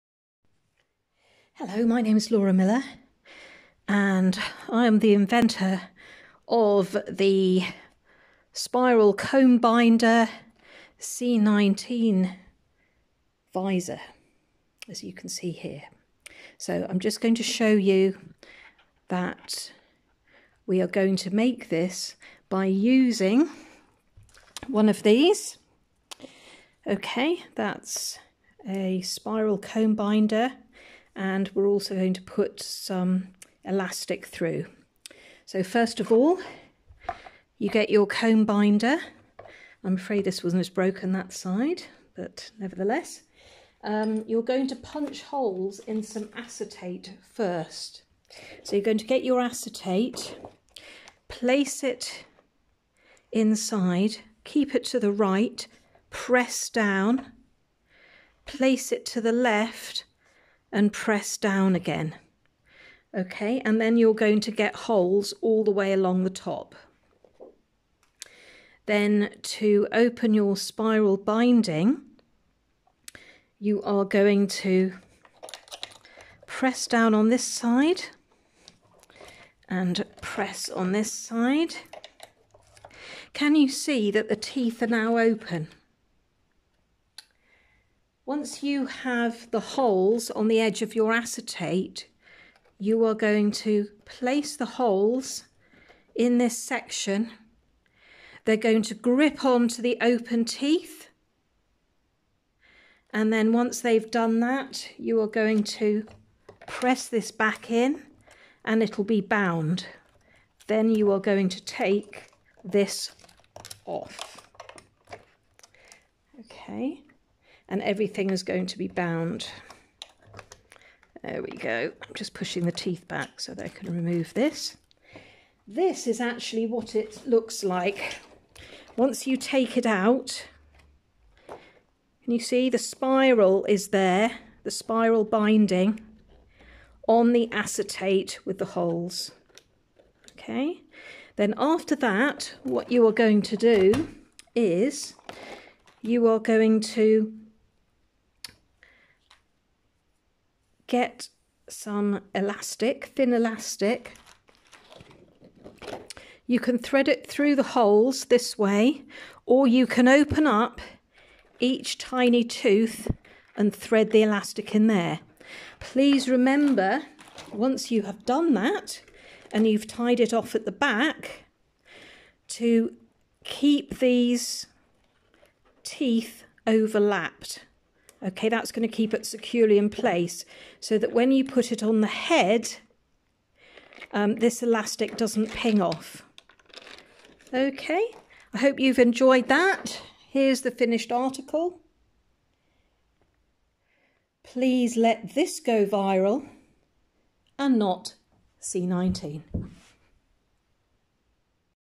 C19 DIY Comb Binder Visor Face Shield Tuition Video
Step by Step Instructions for making a quick, cheap, easy, C19 DIY Comb Binder VISOR/FACE SHIELD for care home workers and home carers looking after coronavirus patients. Help save lives for just £1.50/unit!

If you have a basic comb document binding machine together with plastic spines, acetate overlay front sheets and some thin elastic - you're set! (Unfortunately I had a dodgy old broken binding machine in this video, that I hadn't used for many years - but I still managed to get the job done!).

N.B. Instead of 'threading' the elastic through the comb teeth, you could of course lay the elastic along the 'open plastic spine teeth' whilst in the machine, together with the acetate hole punched sheet, before clamping them shut...

Get a small round lid and trace around the bottom two sharp corners of the acetate. Smoothly cut a curved edge (safer).

I'd love to see pics of your creations and hear feedback on how you got on.